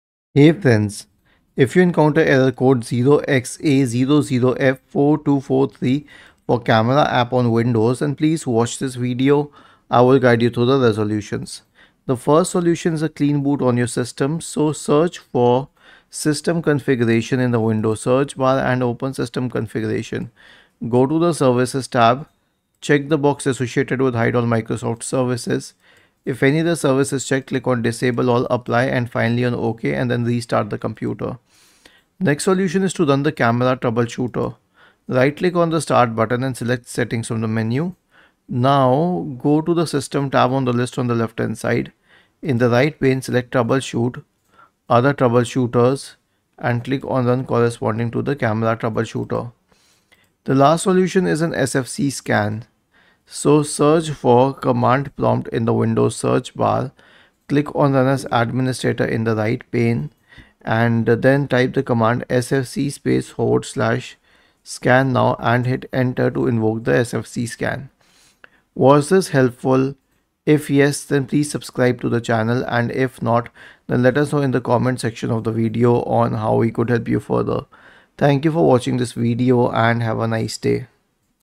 How to Fix Error Code 0xa00f4243 for Camera App on Windows 11
Video Title: How to Fix Error Code 0xa00f4243 for Camera App on Windows 11

If you wish to Error code 0xa00f4243 for Camera app on Windows, then please watch this video.